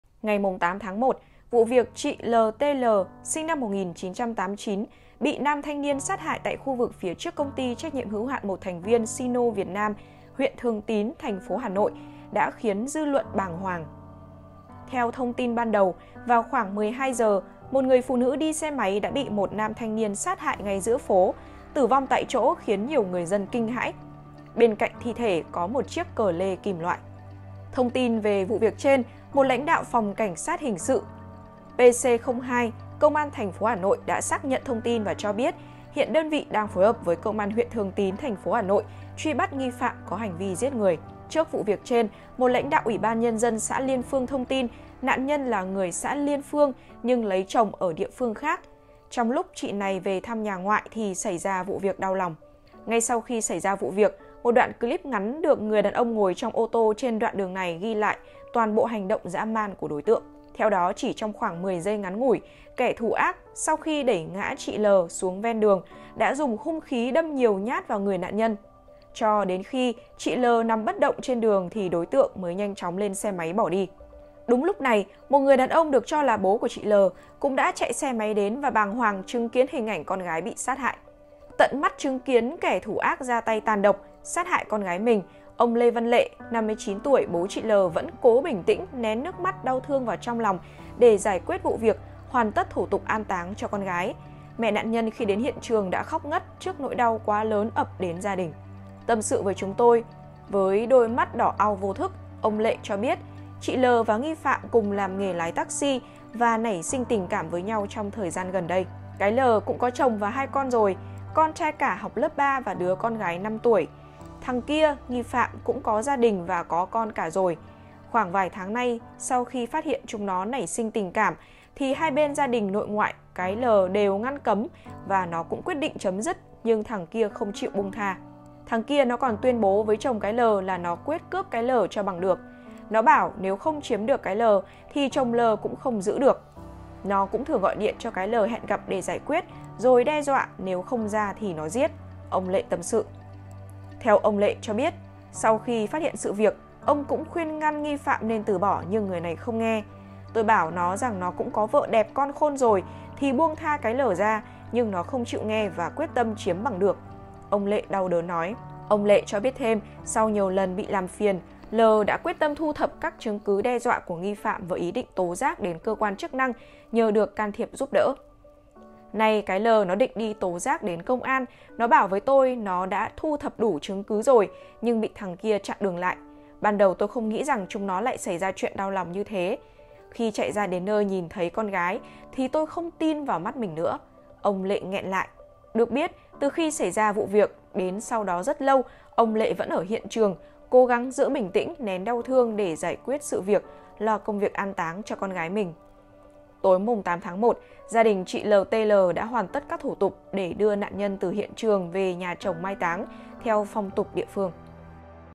Cô gái bị tr.uy s.át t.ử vo.ng giữa phố Hà Nội | Bố cô gái hé lộ nguyên nhân vụ việc
Ngày 8/1, vụ việc chị L.T.L. (SN 1989) bị nam thanh niên sát hại tại khu vực phía trước Công ty TNHH MTV Sino Việt Nam (huyện Thường Tín, TP. Hà Nội) đã khiến dư luận bàng hoàng.
Theo thông tin ban đầu, vào khoảng 12h, một người phụ nữ đi xe máy đã bị một nam thanh niên sát hại ngay giữa phố, tử vong tại chỗ khiến nhiều người dân kinh hãi. Bên cạnh thi thể có một chiếc cờ lê kim loại.